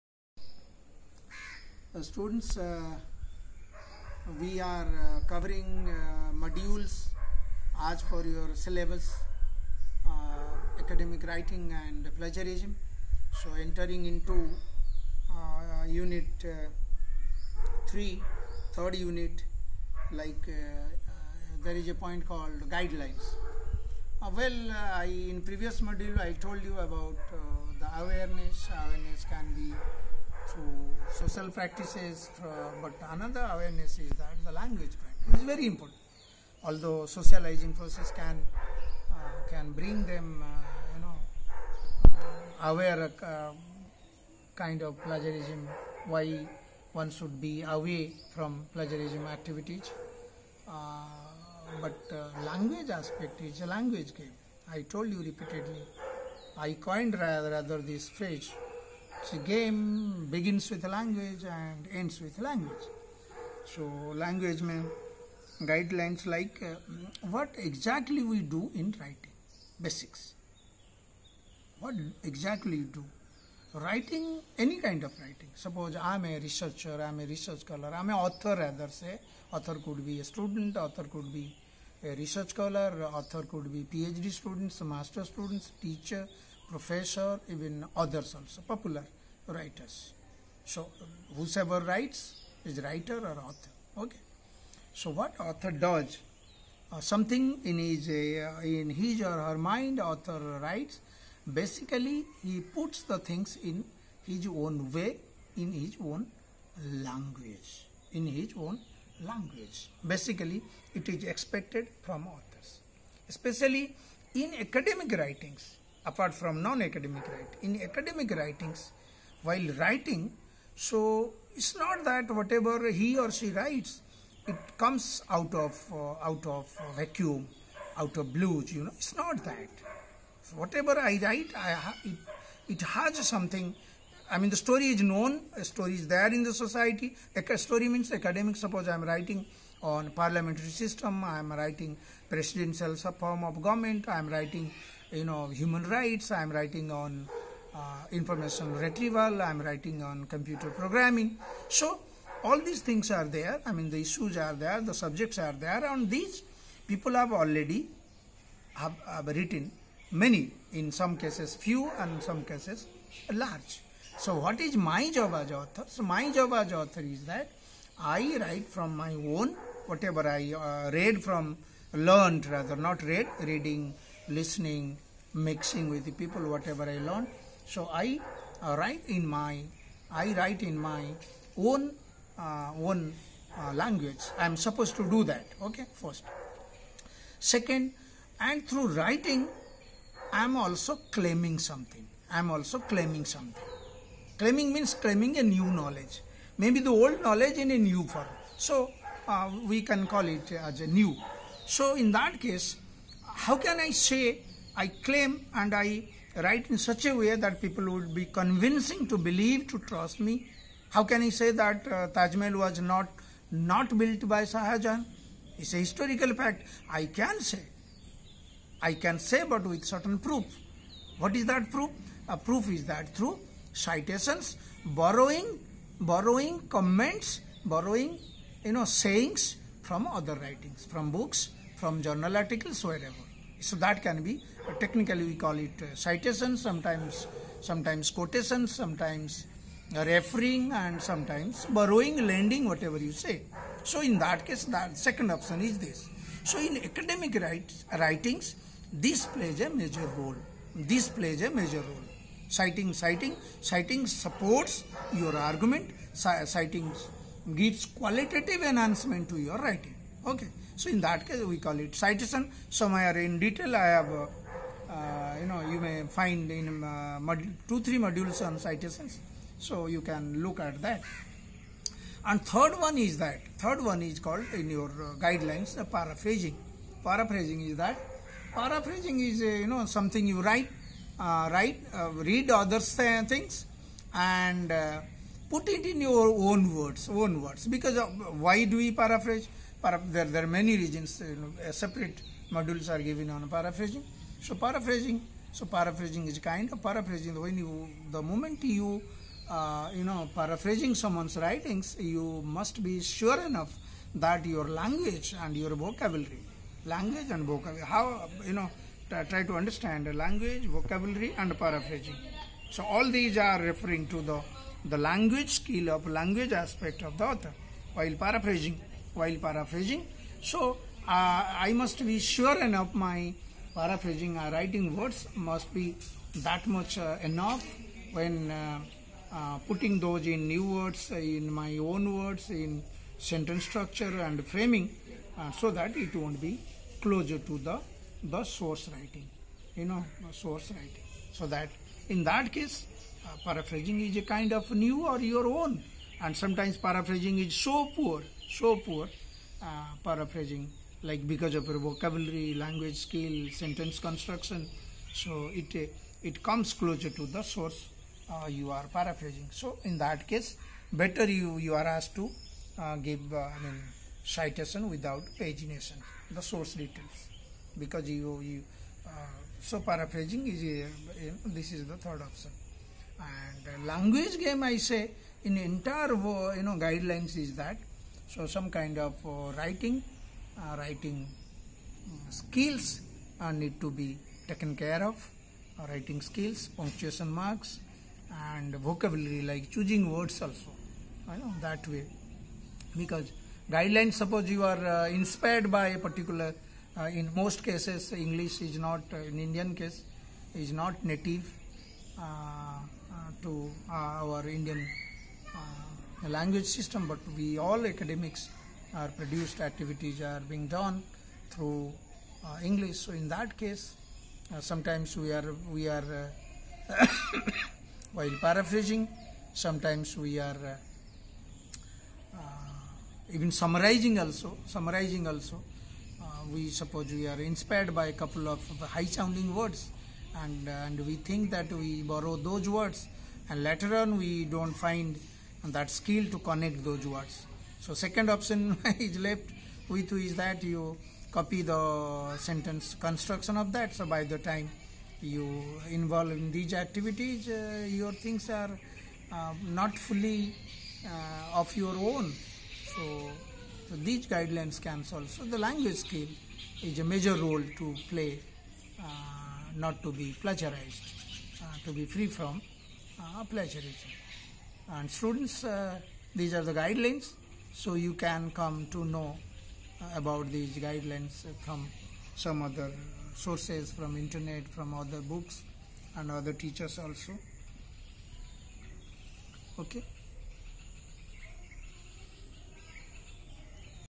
Not to do Plagiarism Guidlines
guidelines for staying away from plagiarism. module speaks in brief as part of the course 'academic integrity and plagiarism' , tught by prof nirmal kumar swain, m d university rohtak who is also speaking in the module